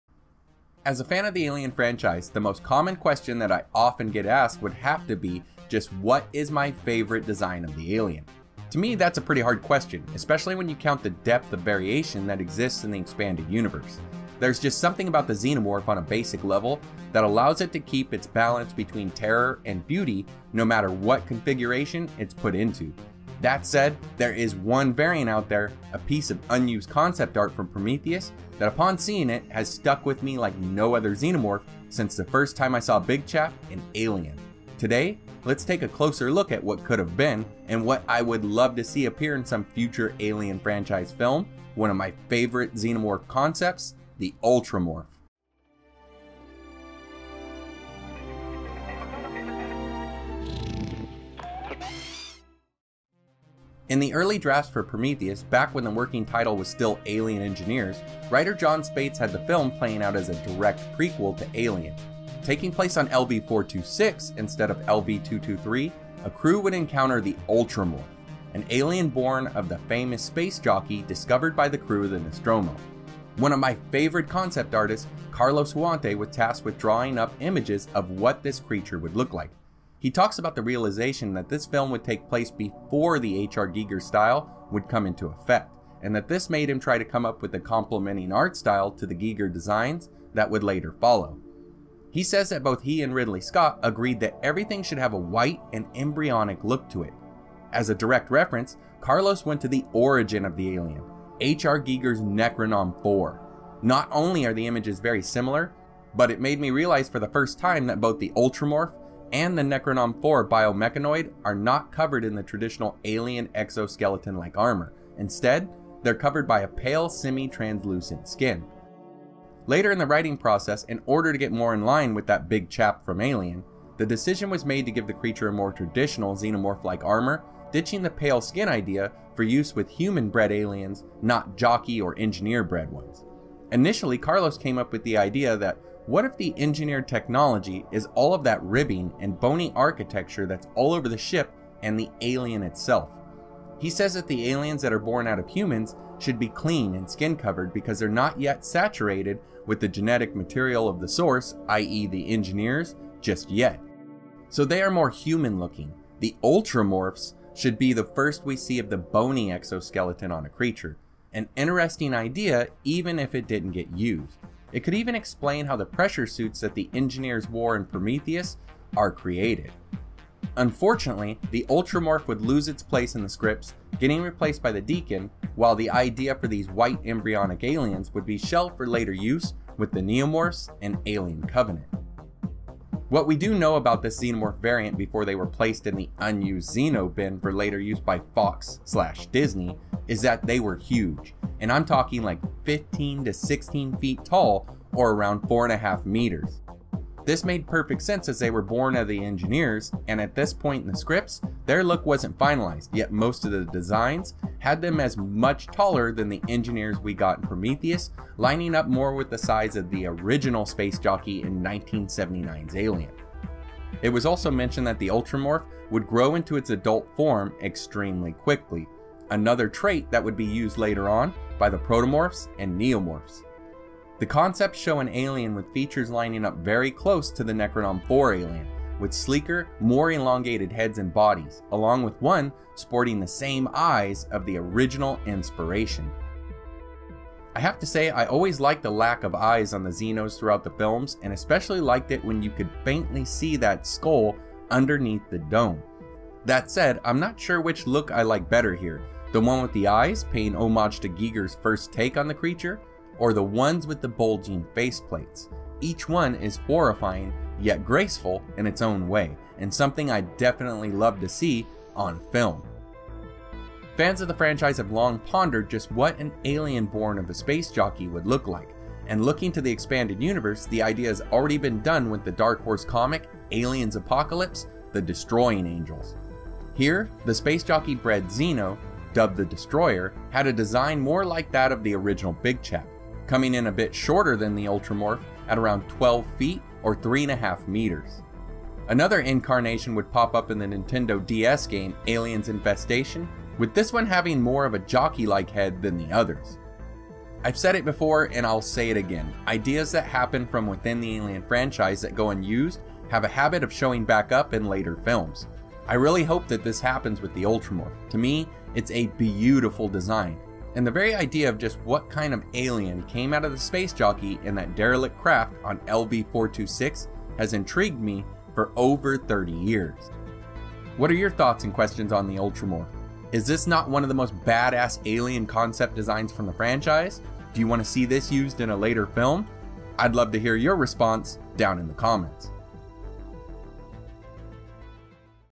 The Ultramorph / 15 Foot Tall Space Jockey Xenomorph / Alien Explained
The ultramorph may just be the pinnacle of xenomorph biology!  Created from a xenomorph and a Space Jockey the creates the Ultramorph!  A 15 to 16 foot tall Alien nightmare!